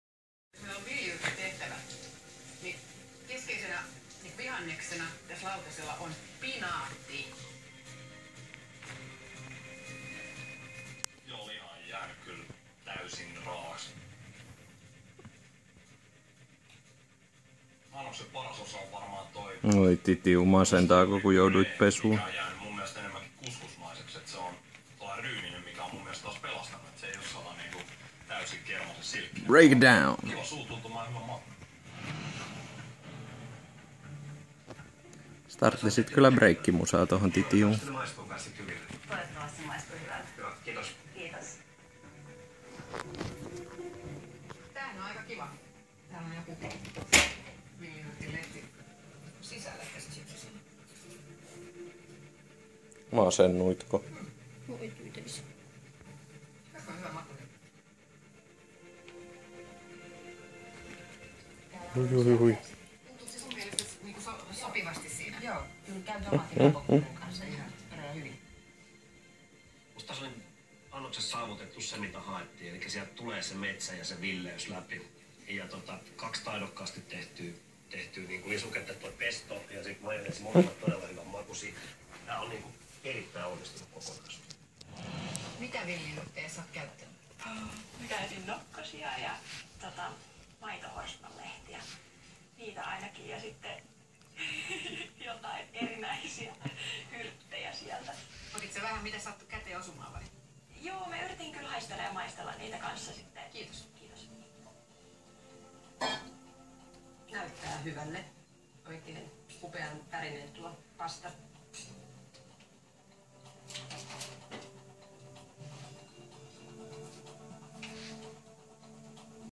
Sheltie having a tantrum after bath | CUTEST REACTION
Again Titiuu got horrified that he had to take a bath. :D